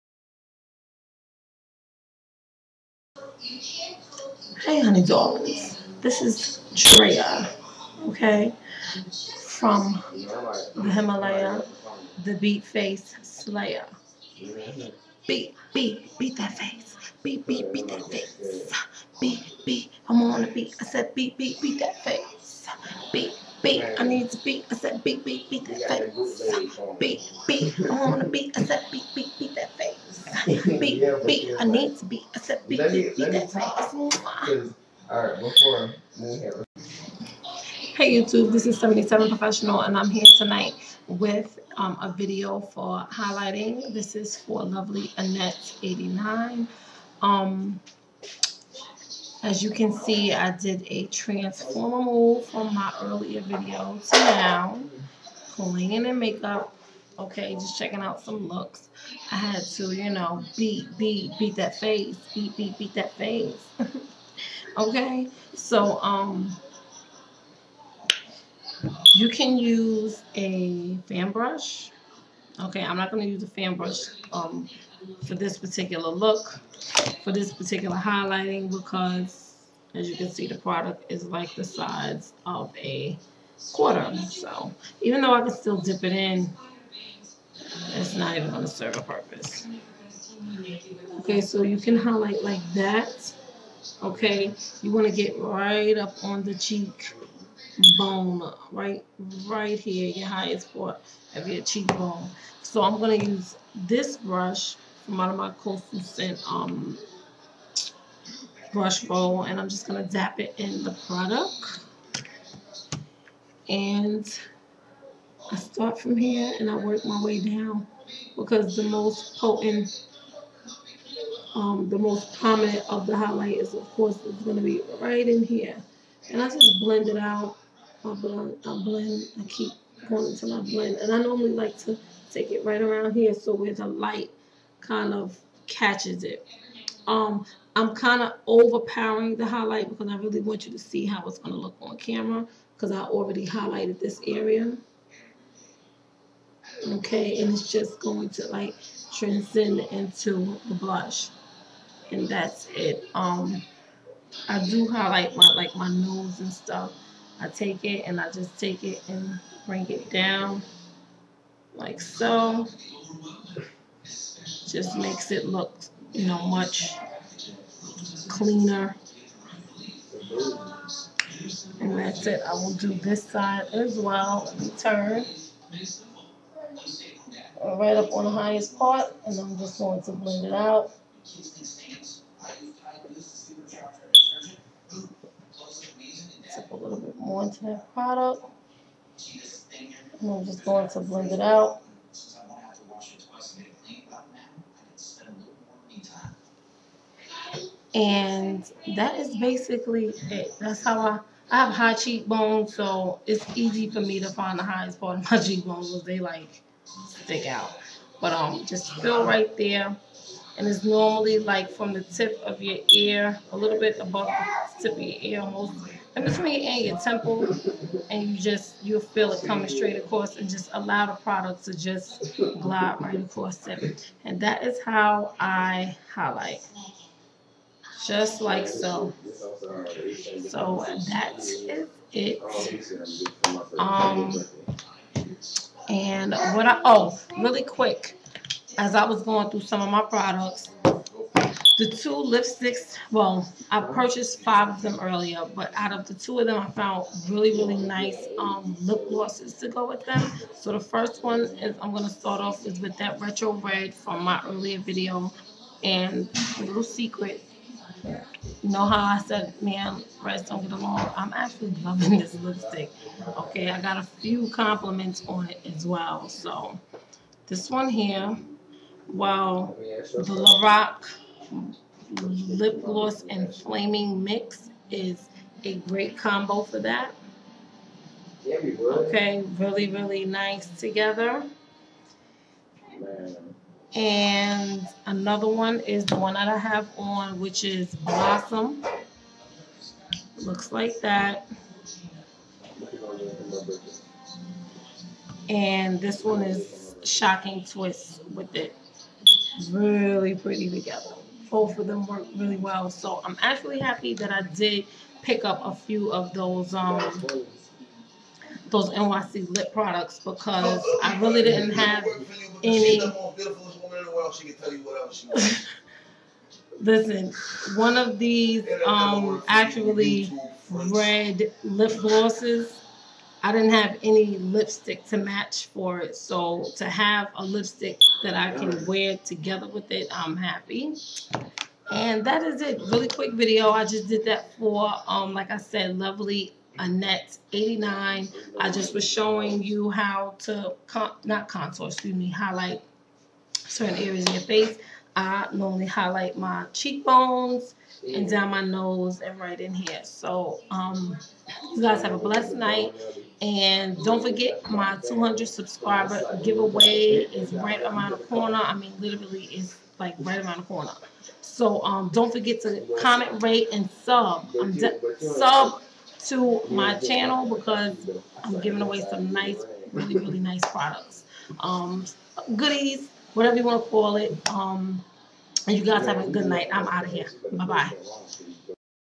Dedication to Drea & Highlighting Face (Requested)
Comment, Rate & SUB 8)

I chose to pay homage to my friend because we awlays would sing that song, when it was time for him to BEAT THAT FACE!

Highlighting video requested by @LovelyAnnette89